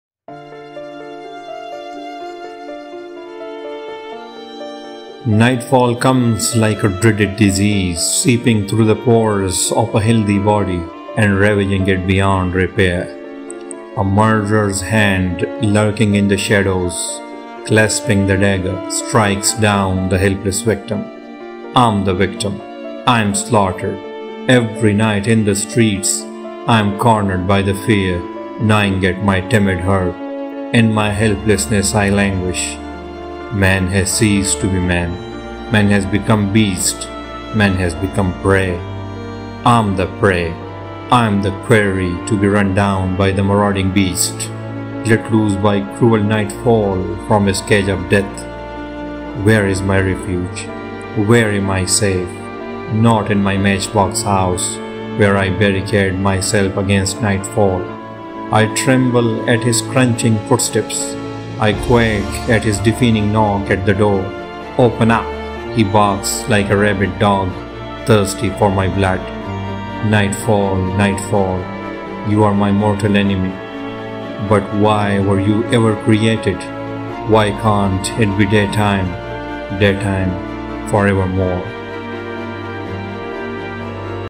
Nightfall in Soweto | A beautiful English poem Recitation
NIGHTFALL IN SOWETO
Nightfall comes like
a dreaded disease
seeping through the pores
of a healthy body
and ravaging it beyond repair

A murderer’s hand,
lurking in the shadows,
clasping the dagger,
strikes down the helpless victim.

I am the victim.
I am slaughtered
every night in the streets.
I am cornered by the fear
gnawing at my timid heart;
in my helplessness I languish.

Man has ceased to be man
Man has become beast
Man has become prey.

I am the prey;
I am the quarry to be run down
by the marauding beast
let loose by cruel nightfall
from his cage of death.

Where is my refuge?
Where am I safe?
Not in my matchbox house
Where I barricade myself against nightfall.

I tremble at his crunching footsteps,
I quake at his deafening knock at the door.
“Open up!” he barks like a rabid dog
thirsty for my blood.

Nightfall! Nightfall!
You are my mortal enemy.
But why were you ever created?
Why can’t it be daytime?
Daytime forever more?

poems of nature and culture
poetry English literature